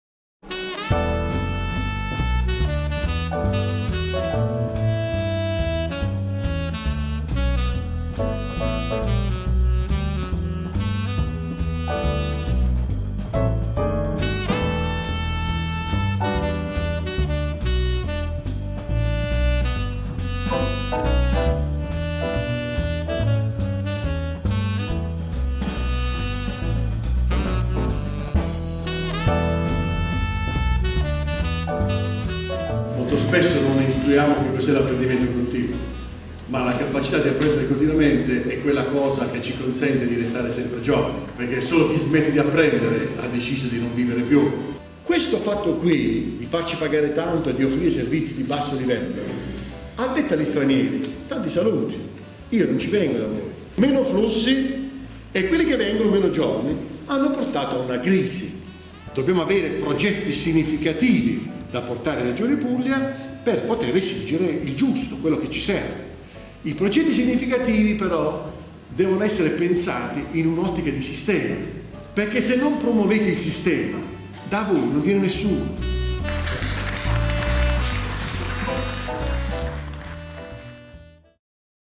A Toritto, per parlare al GAL conca barese del sistema turismo
Per promuovere gli agriturismi e gli alberghi e più in generale il turismo nella zona del Gruppo di Azione Locale della conca barese, occorre fare sistema.